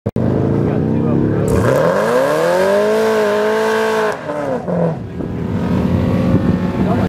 Chevy Camaro Exhaust and Burnout
Well not a burnout, but a practice launch.